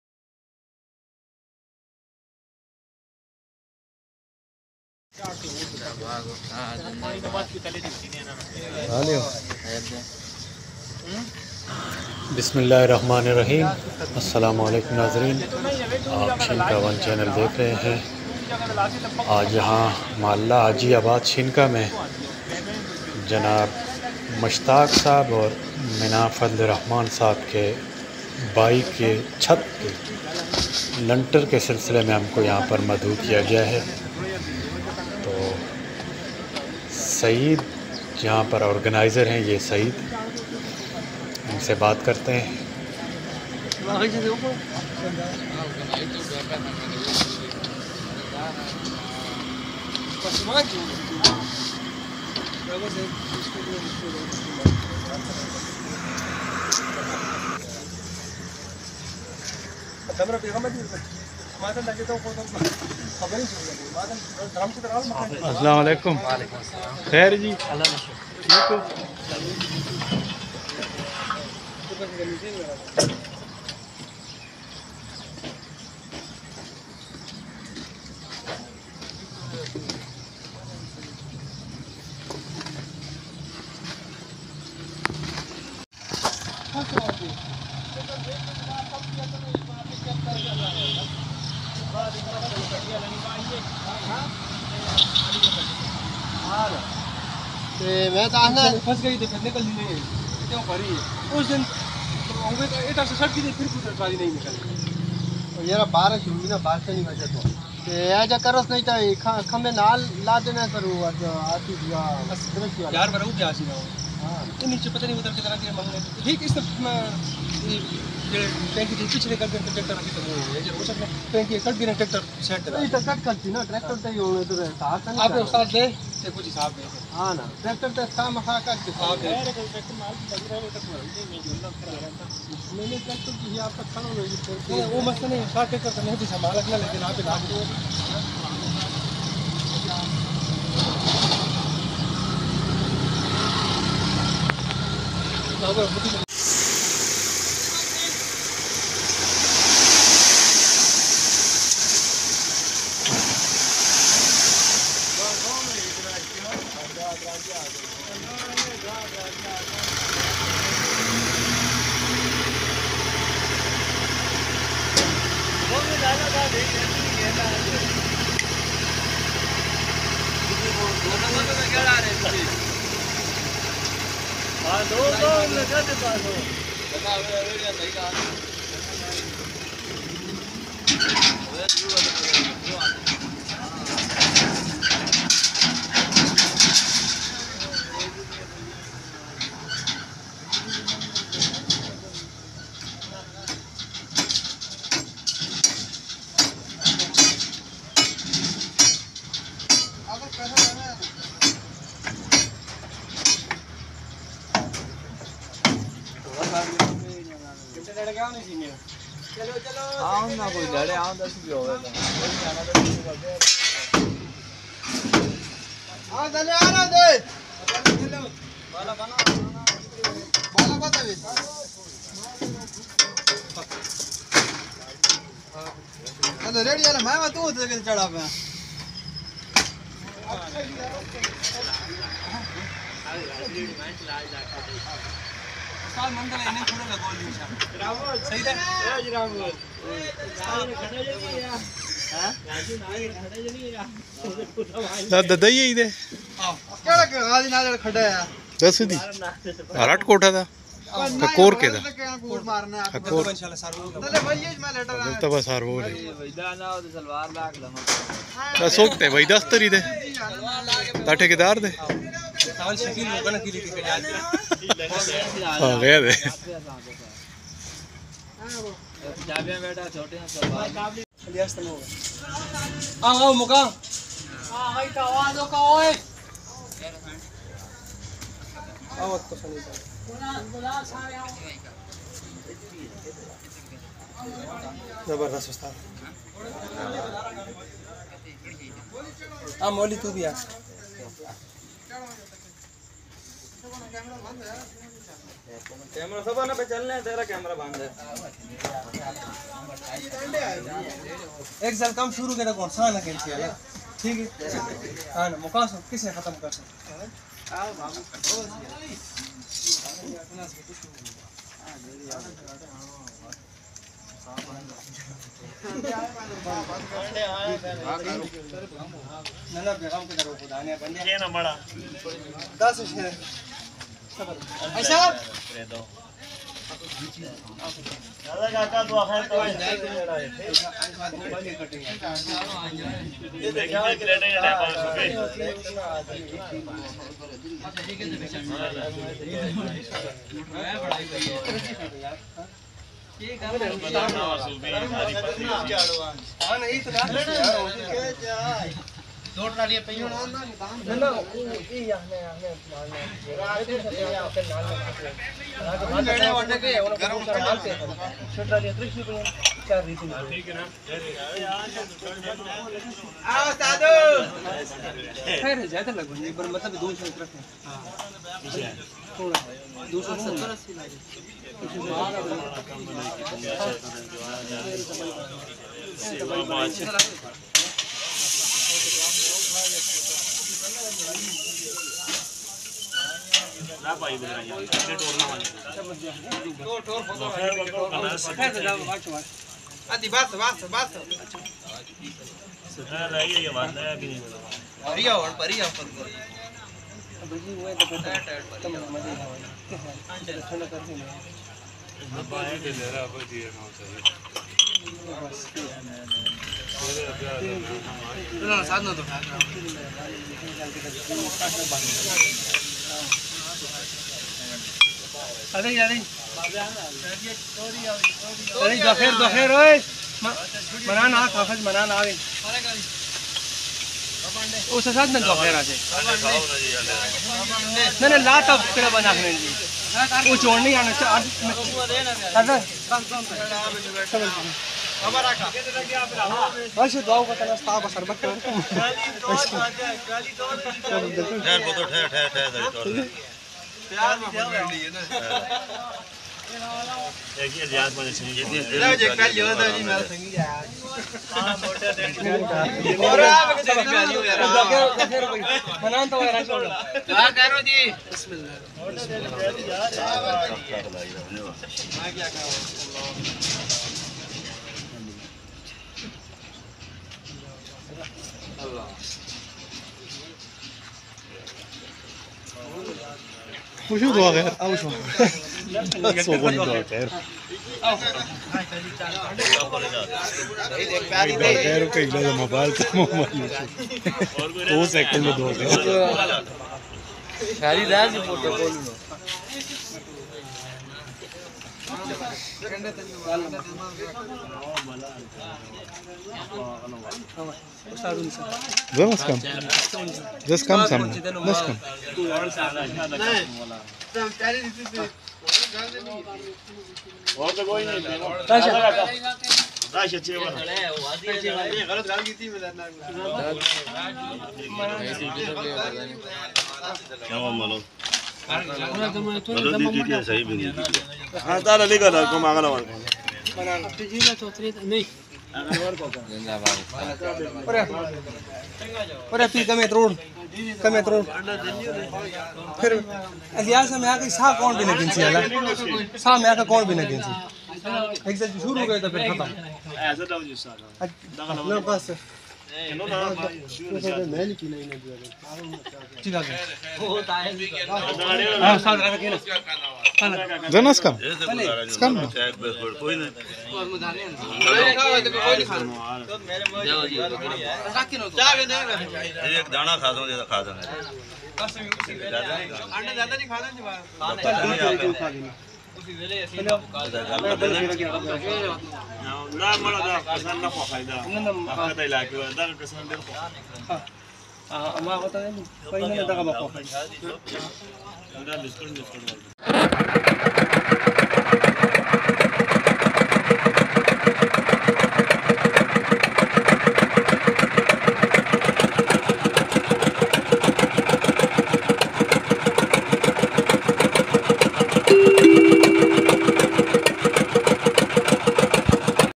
Lanter Saeed khan Shinka
Traditional Construction work of chhachh valley showing in this video l
Dua khair and serving sweets and chai.